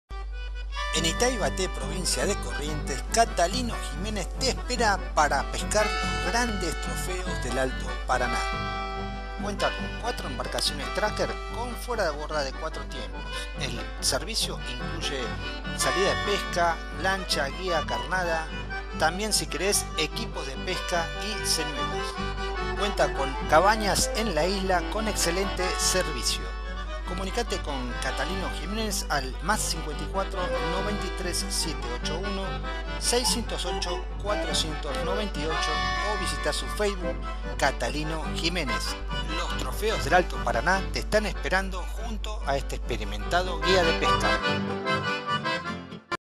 Grandes dorados y surubíes te esperan en Ita Ibaté
Grandes dorados y surubíes te esperan en Ita Ibaté junto al guía Catalino Gimenez, telefono: + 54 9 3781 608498. Facebook: Catalino Gimenez